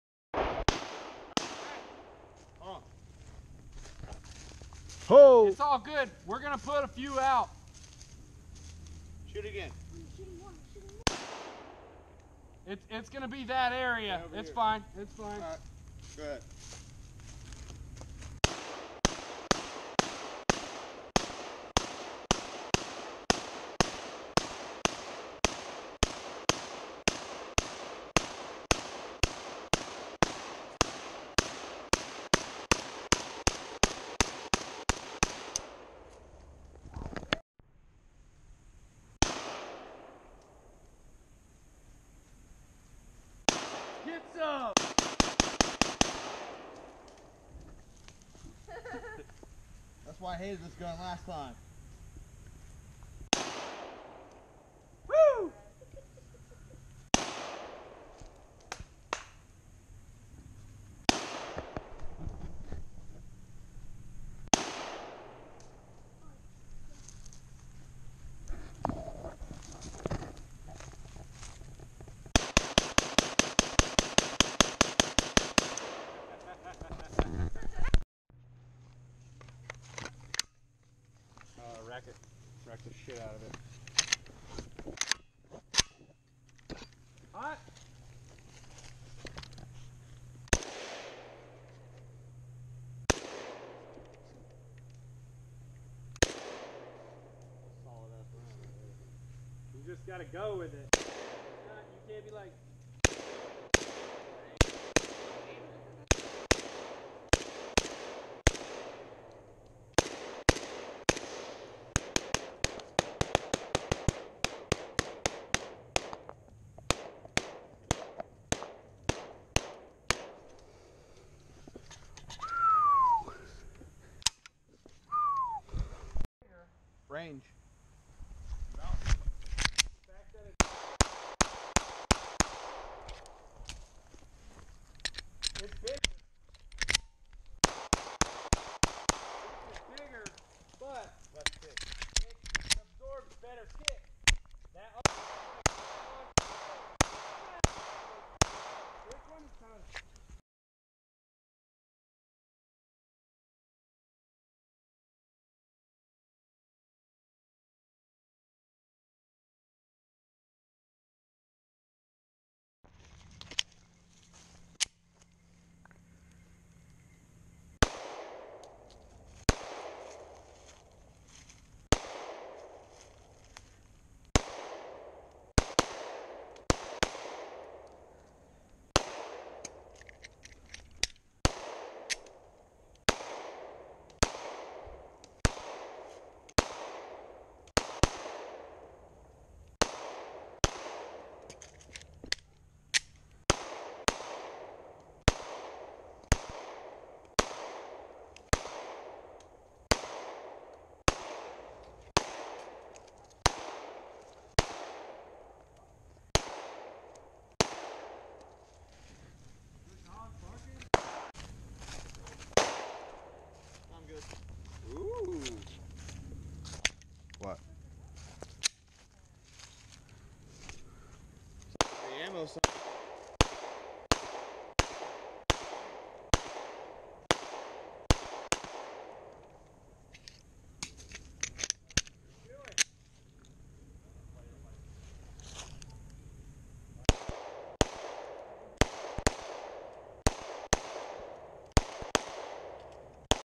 AK 47 and Glock in the scrubs
Welcome to Florida the gun shine state we are shooting 7.62 x 39mm and 9mm ammunition @ shaken up cheap soda cans in a dirt mound from an AK rifle and Glock pistols.